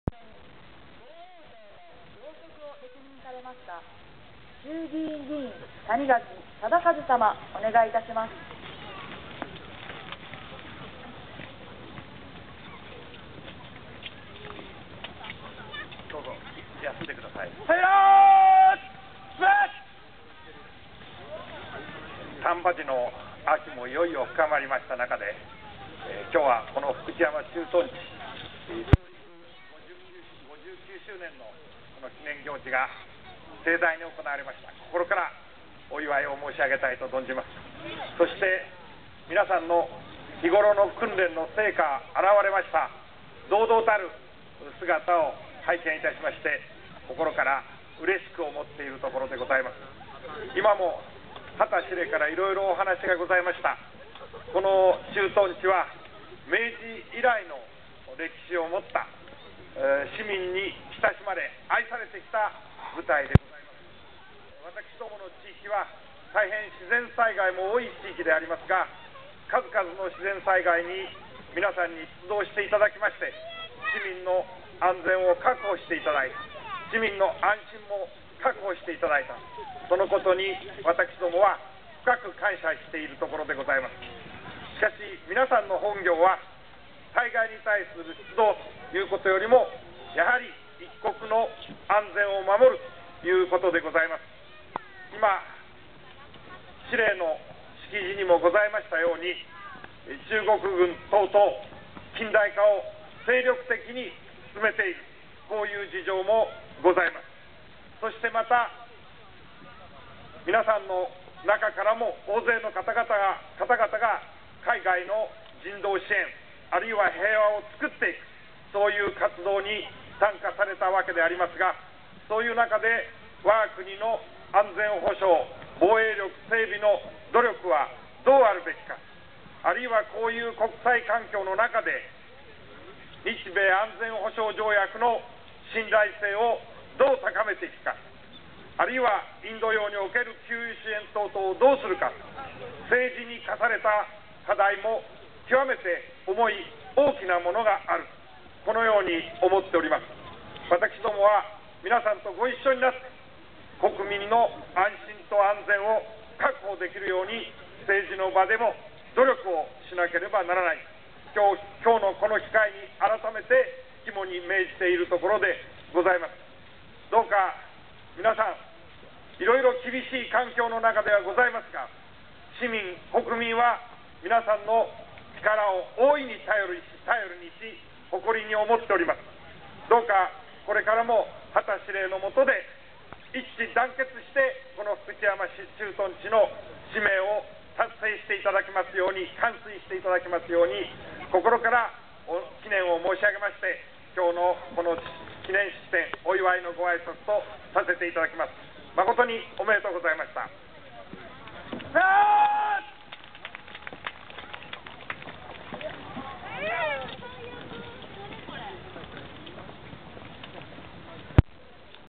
自民党谷垣総裁挨拶
2009年11月28日、福知山駐屯地で行われた記念行事の模様です。自民党の谷垣総裁の演説です。